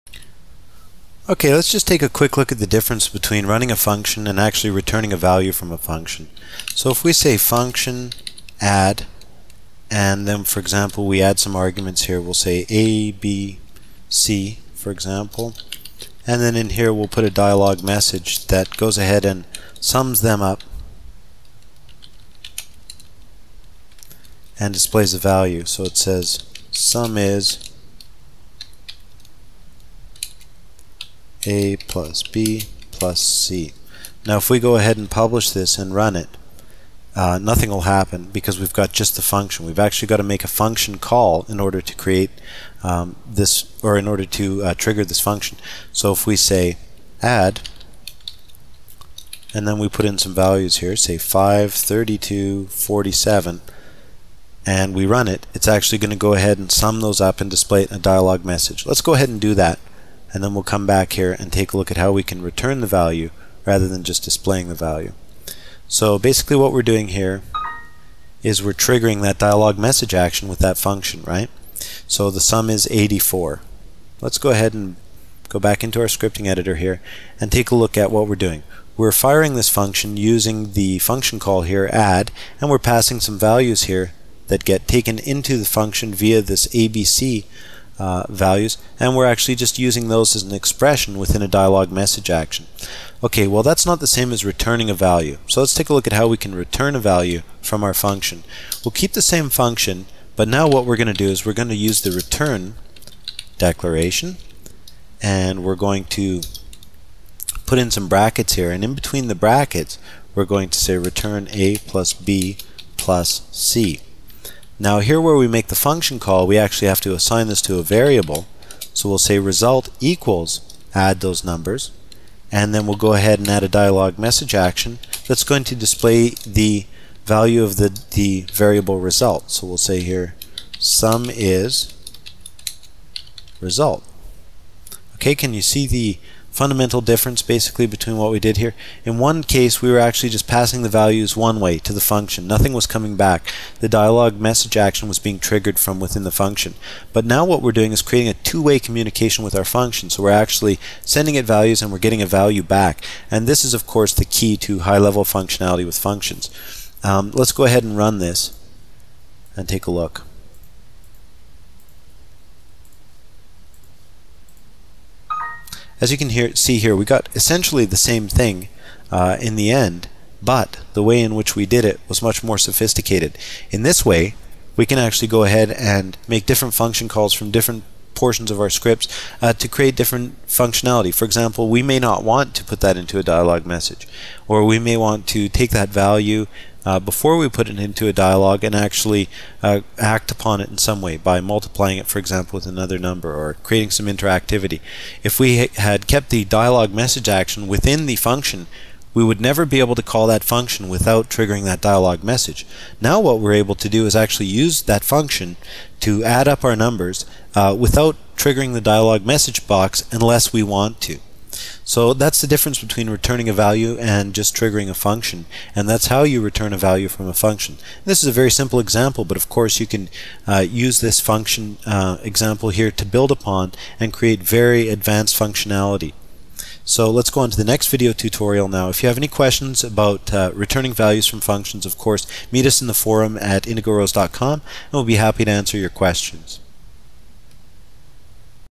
Setup Factory 7 Training Chapter 5 Lesson 12
Actions and Scripting - Returning values from functions